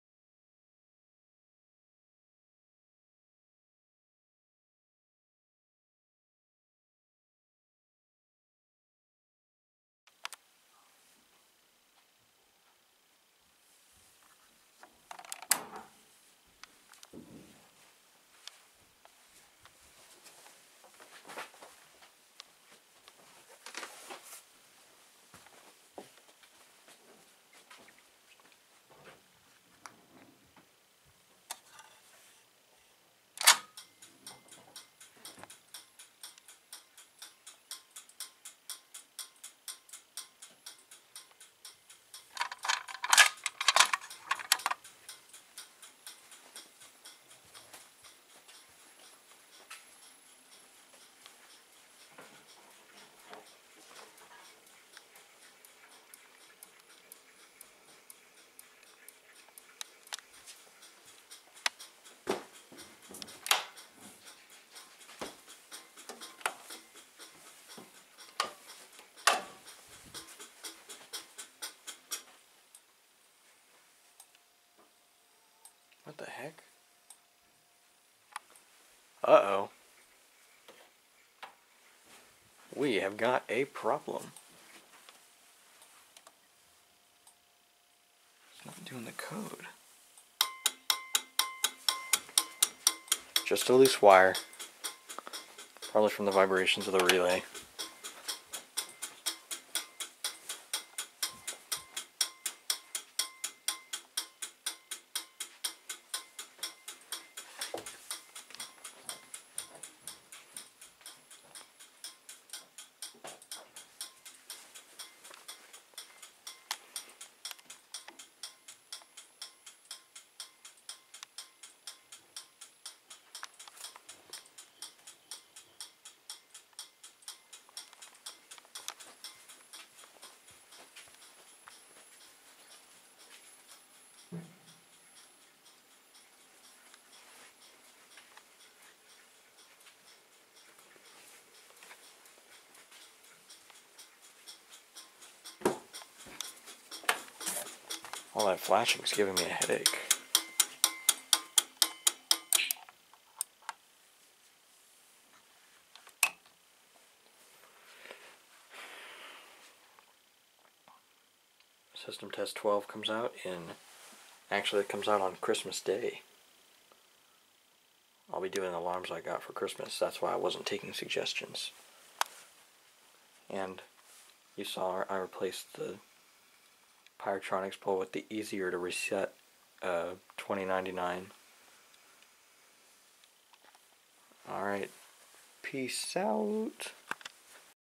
System Test 11.5: Replaced the AV32
I take suggestions for systems.  To see what alarms to suggest,  check the "about" tab on my channel.

ALARMS USED:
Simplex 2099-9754
FCI-MS2
Fire Lite BG-8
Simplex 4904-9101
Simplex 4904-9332
Edwards 892
FOS 6120